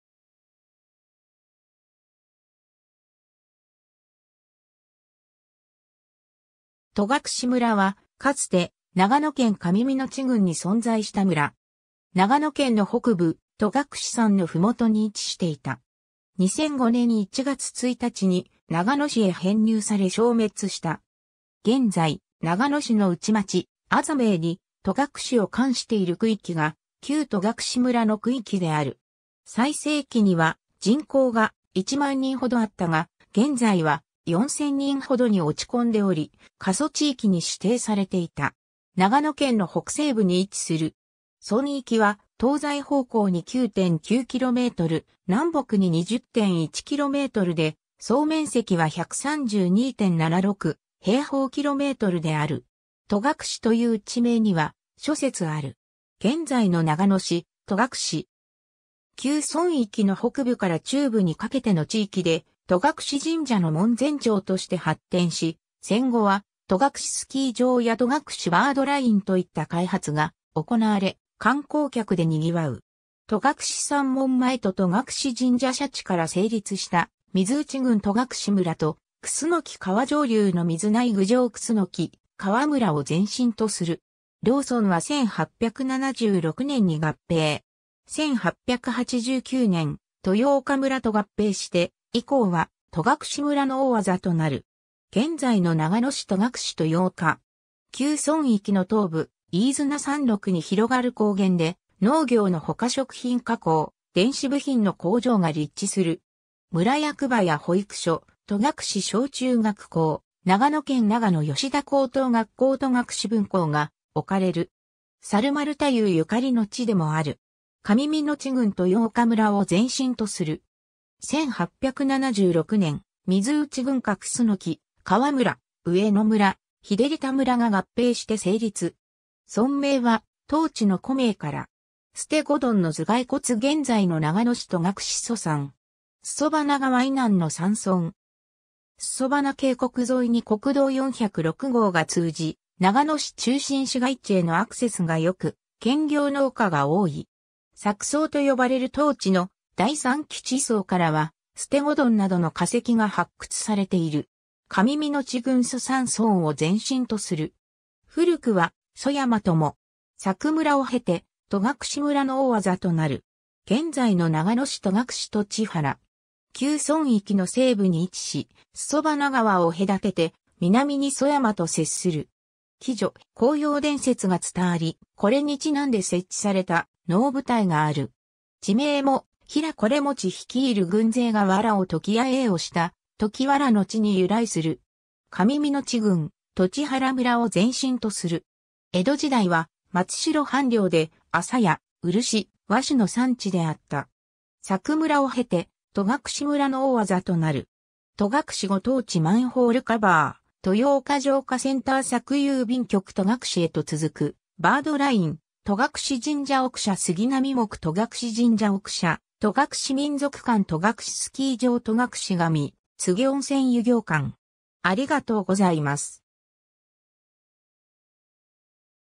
戸隠村
戸隠村（とがくしむら）は、かつて長野県上水内郡に存在した村。
長野県の北部、戸隠山の麓に位置していた。
2005年（平成17年）1月1日に長野市へ編入され消滅した。
現在、長野市のうち町・字名に戸隠を冠している区域が旧戸隠村の区域である。
最盛期には、人口が、10000人ほどあったが、現在は、4000人ほどに落ち込んでおり、過疎地域に指定されていた。
 長野県の北西部に位置する。
村域は東西方向に9.9キロメートル、南北に20.1キロメートルで、総面積は132.76平方キロメートルである。
 「戸隠」という地名には諸説ある。
 現在の長野市戸隠（とがくし、郵便番号：381-4101）。
旧村域の北部から中部にかけての地域で、戸隠神社の門前町として発展し、戦後は戸隠スキー場や戸隠バードラインといった開発が行われ、観光客で賑わう。
 戸隠山門前と戸隠神社社地から成立した水内郡戸隠村と、楠川上流の水内郡上楠川村を前身とする。
両村は1876年（明治9年）に合併。
1889年（明治22年）、豊岡村と合併して以降は戸隠村の大字となる。
 現在の長野市戸隠豊岡（とがくしとよおか、郵便番号：381-4102）。
旧村域の東部、飯縄山麓に広がる高原で、農業のほか食品加工・電子部品の工場が立地する。
村役場（現・長野市役所戸隠支所）や保育所、戸隠小・中学校、長野県長野吉田高等学校戸隠分校が置かれる。
猿丸太夫ゆかりの地でもある。
 上水内郡豊岡村を前身とする。
1876年（明治9年）、水内郡下楠川村・上野村・日照田村が合併して成立（当初は水内郡）。
村名は当地の古名から。
 ステゴドンの頭蓋骨 現在の長野市戸隠祖山（とがくしそやま、郵便番号：381-4103）。
裾花川以南の山村。
裾花渓谷沿いに国道406号が通じ、長野市中心市街地へのアクセスが良く（自動車で30分）、兼業農家が多い。
柵層と呼ばれる当地の第三紀地層からは、ステゴドンなどの化石（約400万年前）が発掘されている。
 上水内郡（旧・水内郡）祖山村を前身とする。
古くは曽山とも。
柵村を経て戸隠村の大字となる。
 現在の長野市戸隠栃原（とがくしとちわら、郵便番号：381-4104）。
旧村域の西部に位置し、裾花川を隔てて南に祖山と接する。
鬼女・紅葉伝説が伝わり、これにちなんで設置された能舞台がある。
地名も平維茂率いる軍勢が藁を解き野営をした「ときわらの地」に由来する。
 上水内郡（旧・水内郡）栃原村を前身とする。
江戸時代は松代藩領で、アサやウルシ（実）、和紙の産地であった。
 戸隠ご当地マンホールカバー 豊岡浄化センター 柵郵便局 戸隠へと続くバードライン（飯綱高原にて） 戸隠神社奥社杉並木 戸隠神社奥社 戸隠民俗館 戸隠スキー場 戸隠神告げ温泉 湯行館